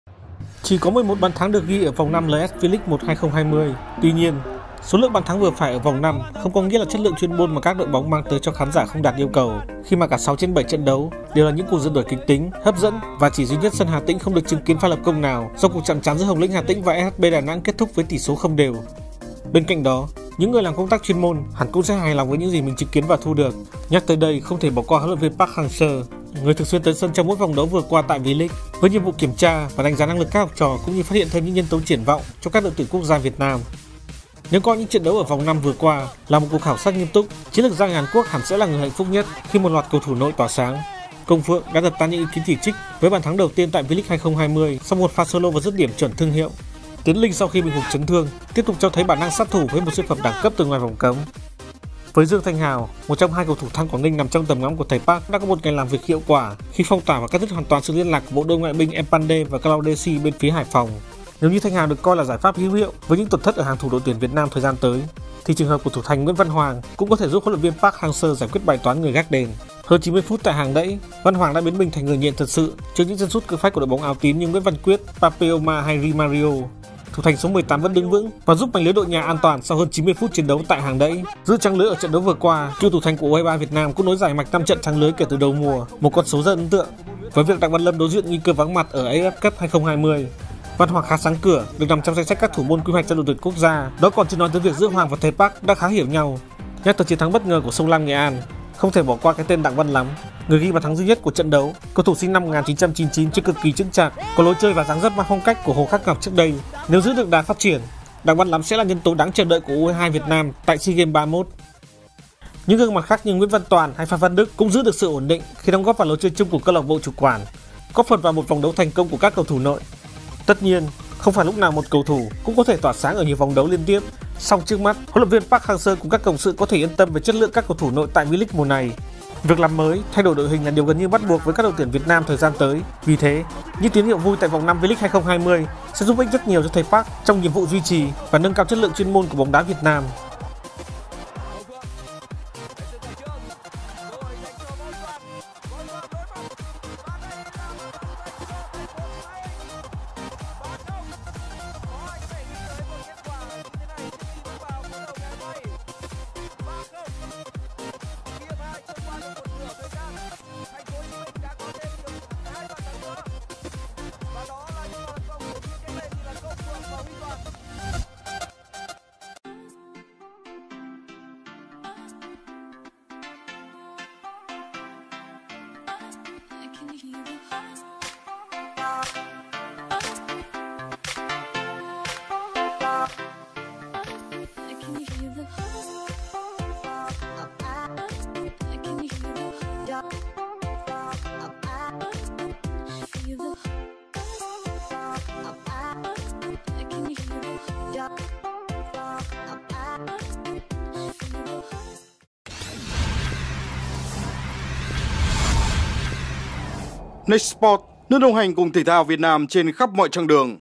Thầy Park bị "cơn đau đầu không hề nhẹ" vì Công Phượng và Tiến Linh | NEXT SPORTS
NEXT SPORTS | Nếu coi những trận đấu ở vòng 5 V.League 2020 là một cuộc khảo sát nghiêm túc, chiến lược gia người Hàn Quốc hẳn sẽ trải qua "cơn đau đầu dễ chịu" vì có nhiều sự lựa chọn trên hàng công.